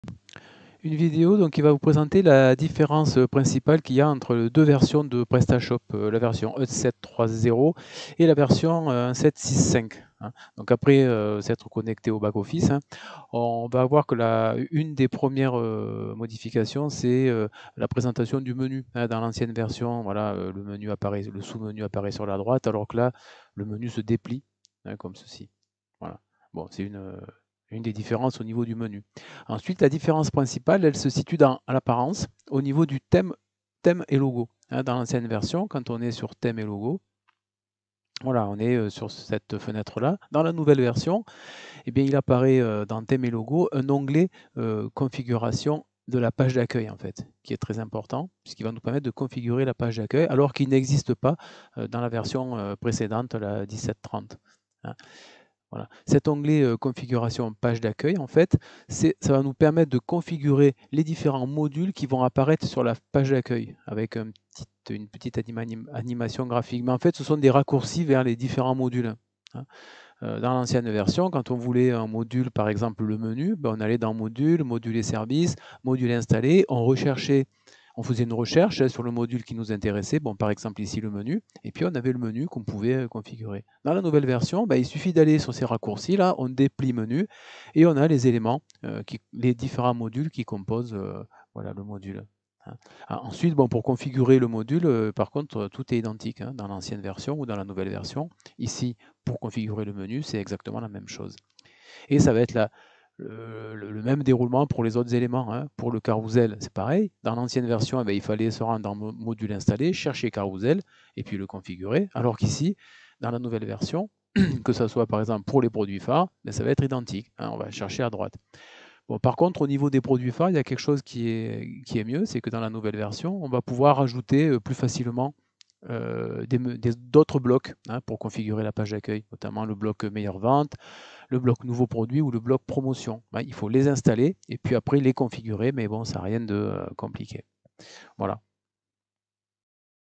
Différence entre les Versions de Prestashop
BTS NDRC, Prestashop différence entre les versions de Prestashop
00:28 : l'onglet apparence-Thème logo